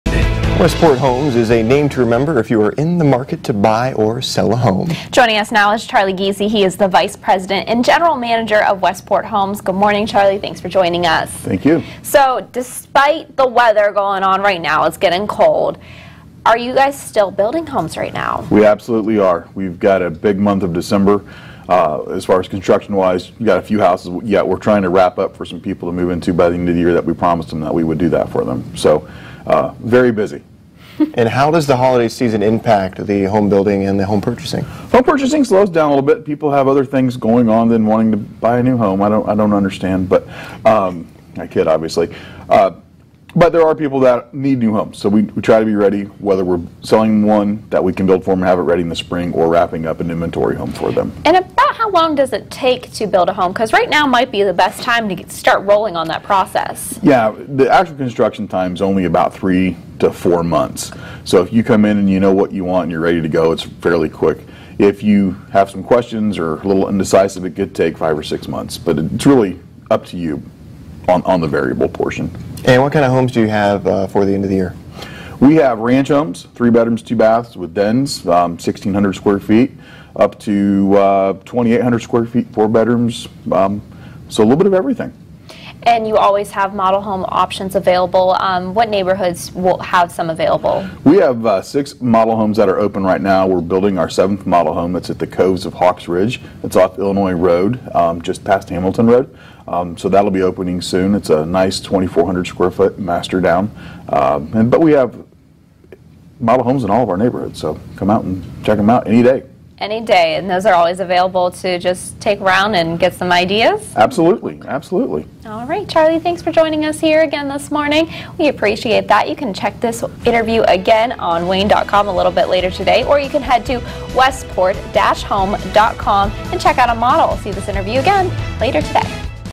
Sunday Open House December edition
Just because it's getting colder, doesn't mean you can't build a home. Westport Homes explains what option are available.